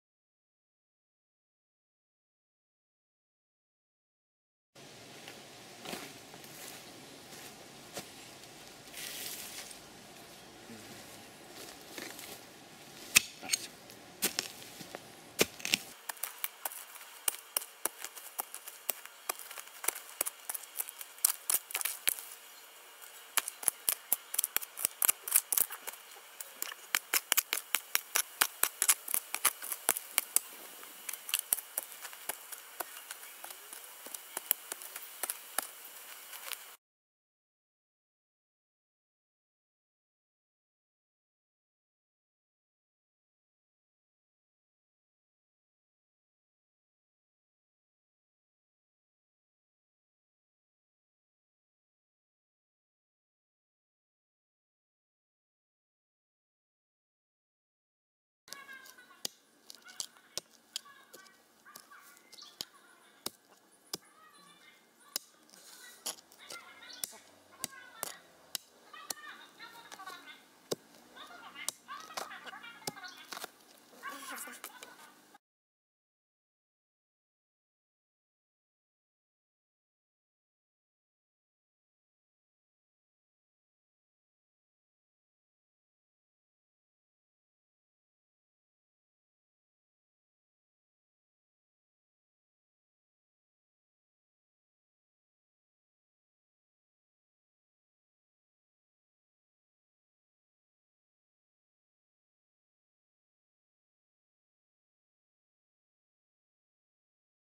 タケノコに苦戦 1 ～早送り
撮影をオートにしてなかったので
雲が出た時のシーンが真っ暗・・・。

４月上旬、この辺りでは今がピークでしょうか？